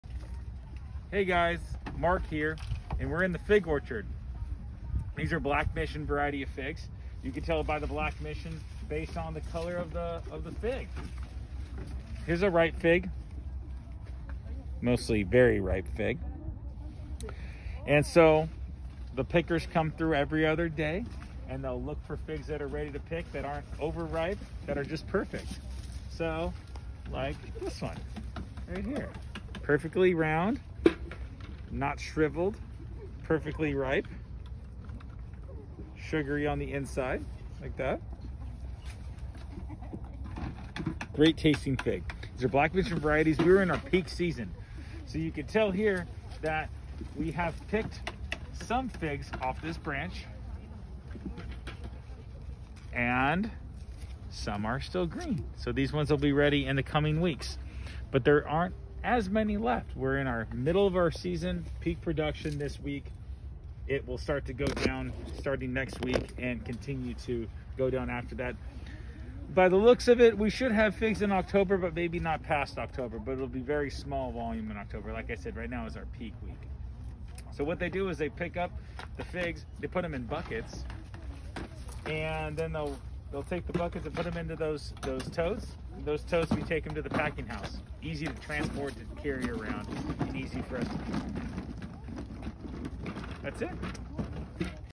Fig Crop, August 2020
Watch Farmer Marc explain for Fig crop in Le Grand, CA.